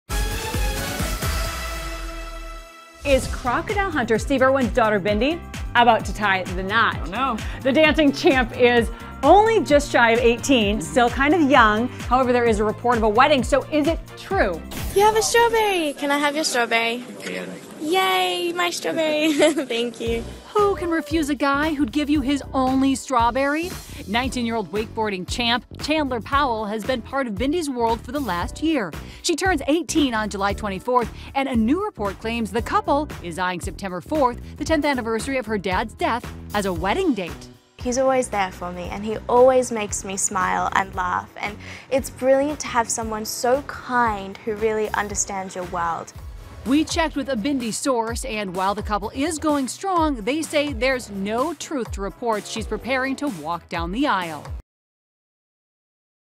Um... Is Bindi Irwin Getting Married When She Turns 18?
The 17-year-old 'Dancing With the Stars' champ seems to be in love, but a source says she's not ready to walk down the aisle just yet.